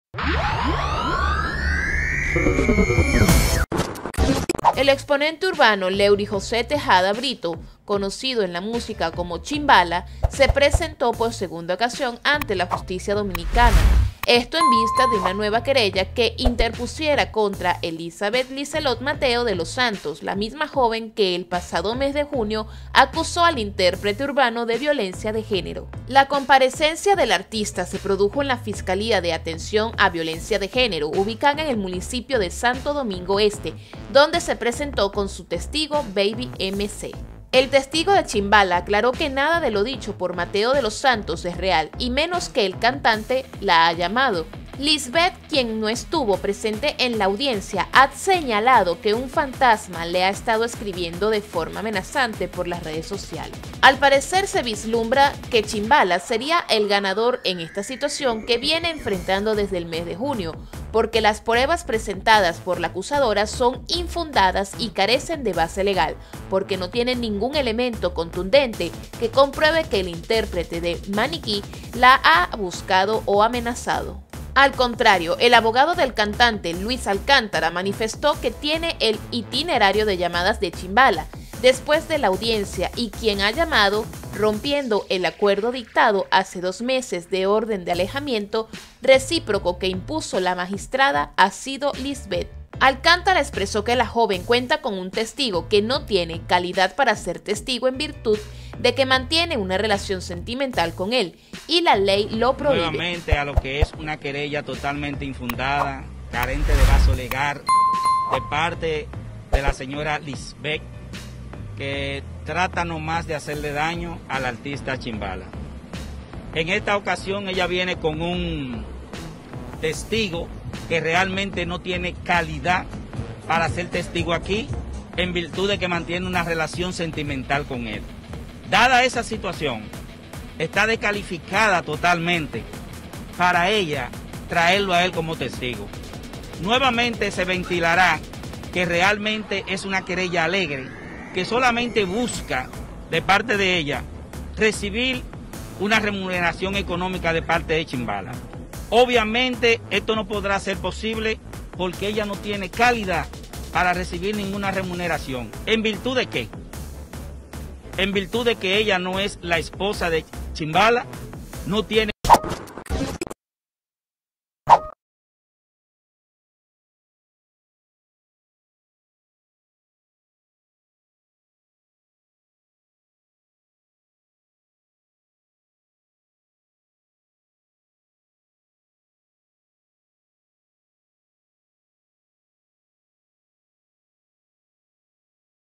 🚨 Chimbala se presentó en la justicia nuevamente; te contamos lo que allí sucedió 🚨
🚨 MundoRdTv 🚨